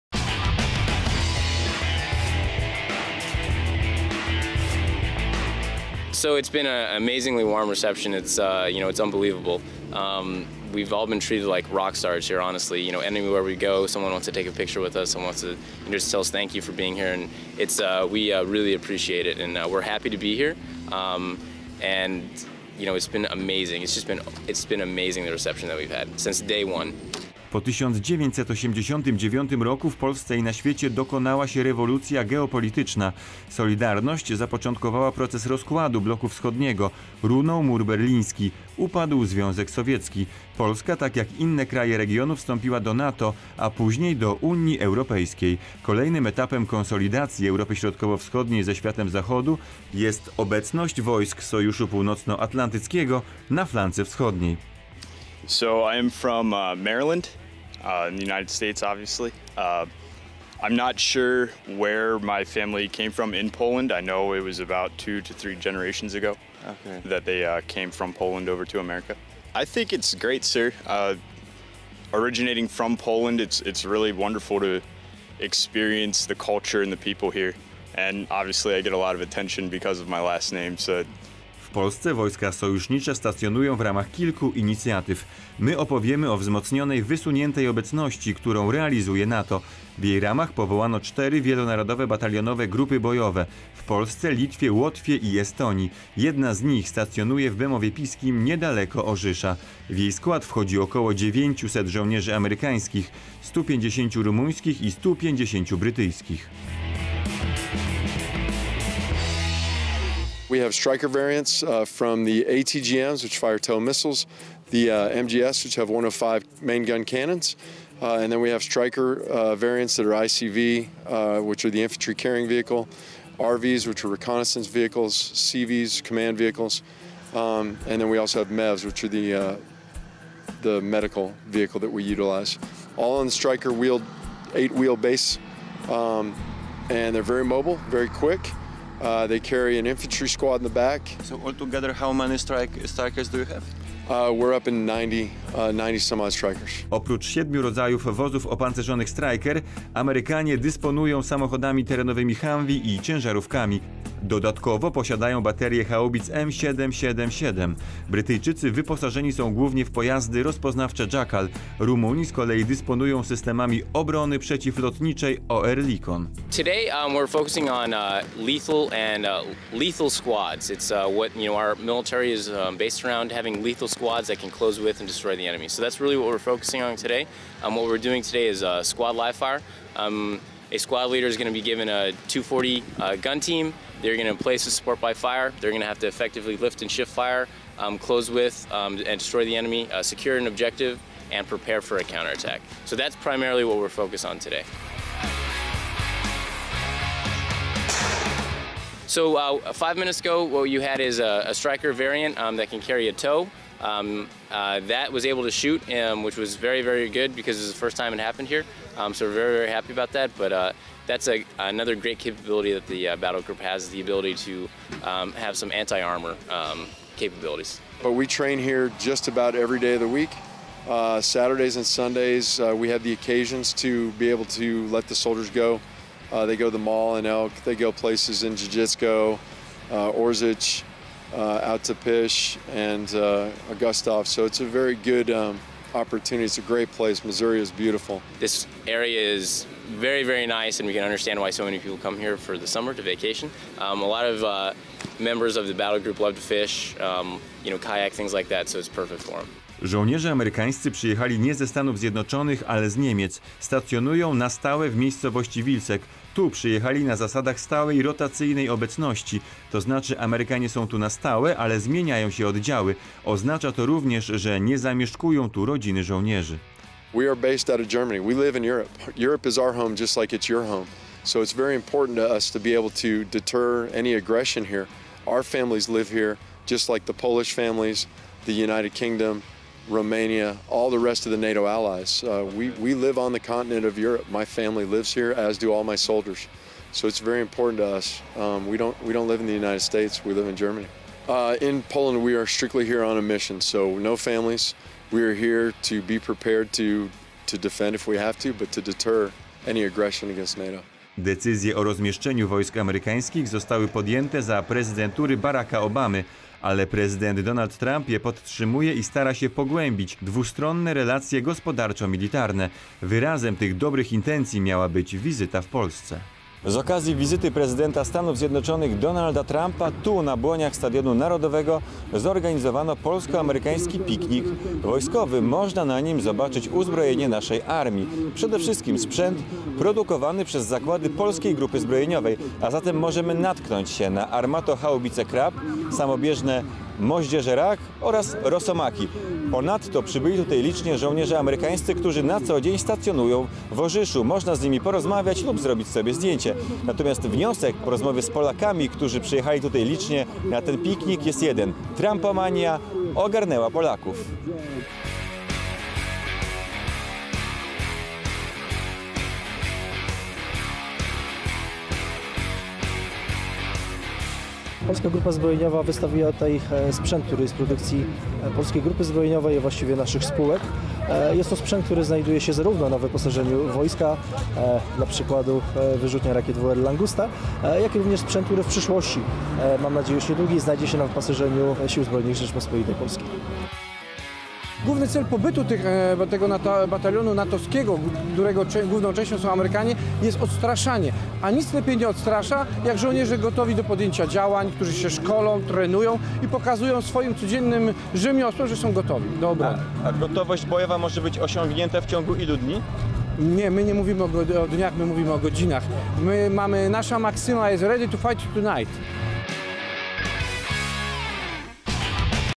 TV RepublikaExcerpt
(Capt. Montoya speaking)
Reporter: After 1989 in Poland many things has changed. The Berlin Wall came down, The Soviet Union collapsed, Poland joined the NATO and European Union. Consolidation of eastern Europe with the western world began.
(Sgt. Zaczek speaking)
Reporter: There is more than one reason why the American Army is present in Poland. NATO has deployed big battle groups in eastern Europe. They are staying in Poland, Lithuania, Latvia and Estonia. The biggest group is the American battle group which is stationed in Bemowo Piskie, close to Orzysz. It has over 900 Americans, 150 British and 150 Romanians.
(Lt. Col. Gventer speaking)
Reporter: The unit has 7 different kinds of Strykers. Americans also have humvees and LMTVs. In addition, they have howitzers batteries M777. The British have recon vehicles called Jackals and the Romanians have Oerlikon anti-aircraft guns.
(Capt. Montoya and Lt. Col. Gventer speaking)
Reporter: American soldiers came here not from the USA, but from Germany--they are stationed in Vilseck. It means they are permanently based in Europe but their squadron has deployed to Poland as part of the battle group.
Reporter: The decision of U.S. Soldiers coming to Europe was made by Barrack Obama. President Donald Trump is continuing this and trying to develop relations between USA and Europe. Because of the visit of Donald Trump in Poland, we also had a military picnic in front of the National Stadium, where people could see Polish military equipment. American, Romanian and British soldiers from the battle group stationed in Orzysz also came with some equipment, so people could talk with them and take photos. Many people came especially to Warsaw to see the American president, we can talk about Trump-mania. Polish PGZ also have a lot of military equipment on the static display. We have WR-40 Langusta which is a rocket launcher.
15th Mechanized Brigade commander Gen. Jaroslaw Gromadzinski: The main reason why we have this big battalion in Orzysz is to scare away the enemy because there is nothing scarier than all of a battle group ready and prepared to fight.
Reporter: How long does it take to achieve readiness for all the soldiers?
Gen. Gromadzinski: We don't talk about days or hours. We always say "ready to fight tonight.”